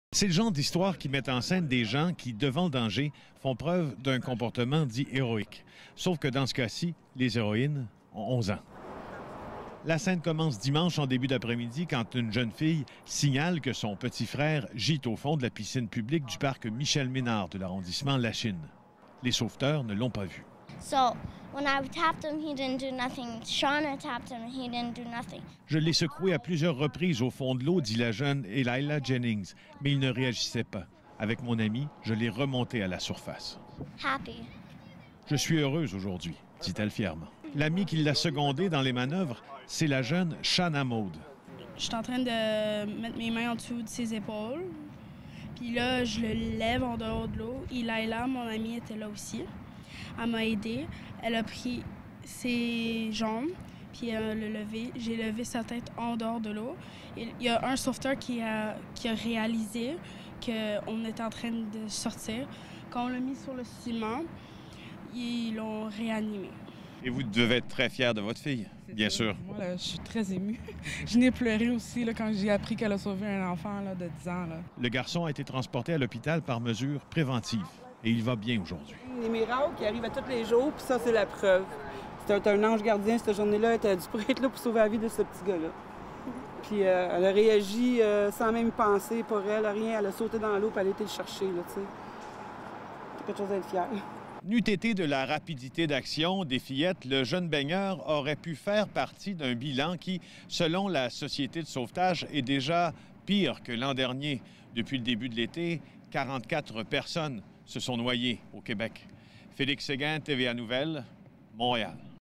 Une jeune fille de 11 ans sauve la vie d'un garçon de 10 ans qui se noyait
Une jeune fille de 11 ans a sauvé de la noyade un garçon de 10 ans la fin de semaine dernière dans une piscine publique de l'arrondissement Lachine, à Montréal.

La piscine du parc Michel-Ménard était bondée dimanche lorsqu’aux environs de 13h40 un jeune de 10 ans qui a échappé à la vigilance des sauveteurs s’est retrouvé au fond de l’eau.

C’est Shana-Maude Curadeau qui a retrouvé le garçon et a aidé à ce qu’on le sorte de sa fâcheuse position.

«On était juste en train de jouer à un jeu et la sœur du petit garçon m’a demandé "Est-ce que tu peux aller chercher mon frère au fond de l’eau?" Je pensais que c’était une blague, je vois au fond de l’eau, je touche au petit garçon et il ne ressent rien», raconte la jeune héroïne.

«Je suis en train de mettre mes mains en dessous de ses épaules, je le lève en dehors de l’eau. Mon amie était là aussi, elle m’a aidé, elle a pris ses jambes et j’ai levé sa tête en dehors de l’eau et il y a un sauveteur qui a réalisé qu’on était en train de le sortir», ajoute Shana-Maude.

Les sauveteurs ont réussi à réanimer le garçon. Mais sans l’intervention de Shana-Maude qui sait ce qui aurait pu se produire.

«Je me sens fière heureuse qu’il ait survécu», de dire la jeune fille.

Il y a un même sentiment de fierté chez sa mère qui se félicite d’avoir inscrit sa fille à des cours de natation aussi jeune qu’à un an.

«Dès qu’on commence au plus jeune âge des cours de natation ou de secourisme, ça vaut la peine», explique sa mère Marie-Ève Curadeau qui a pleuré après avoir appris ce qui s’est passé.

L’enfant de 10 ans a été transporté au centre hospitalier par sécurité et se porte bien, a confirmé le Service de police de la Ville de Montréal.